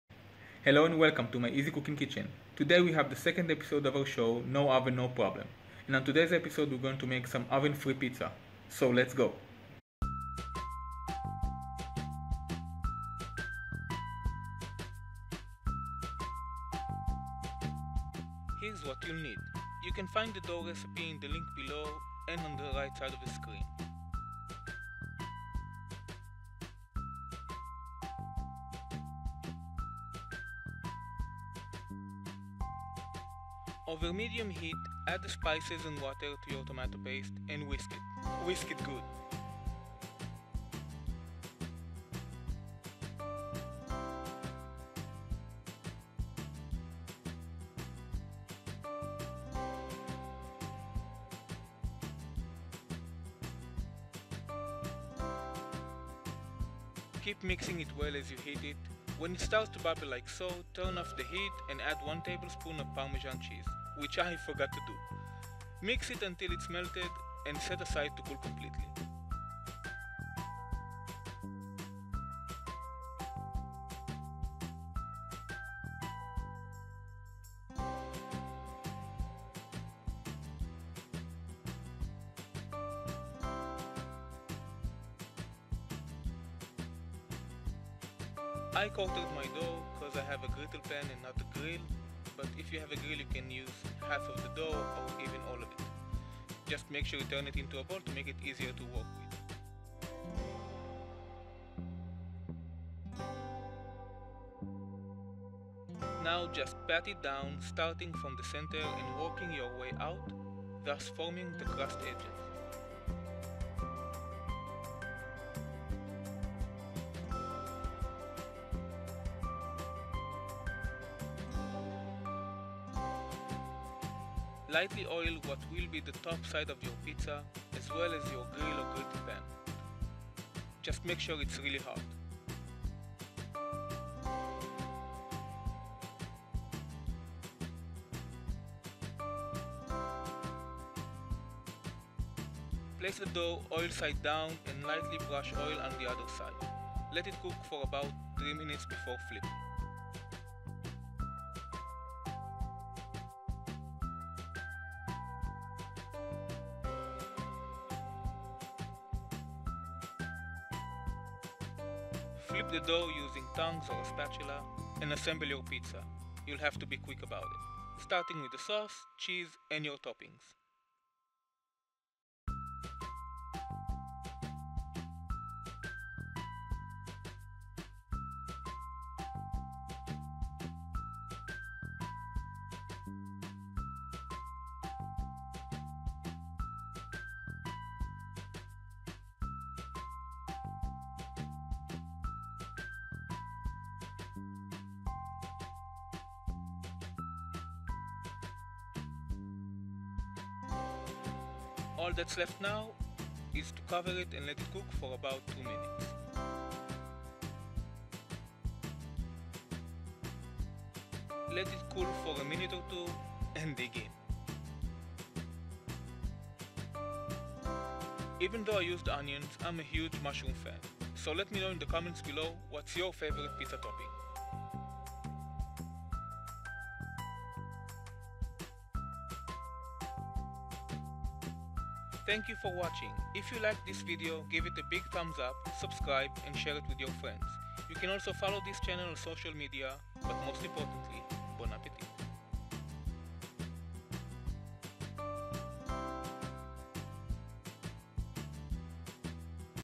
How To Make Homemade Pizza | No Oven = No Problem Ep. #2 | 07.2018
Today i'll show you how you can make an amazing pizza even if you don't have an oven or don't want to turn it on just for that.
This is a simple pizza so let me know what toppings you used on yours :)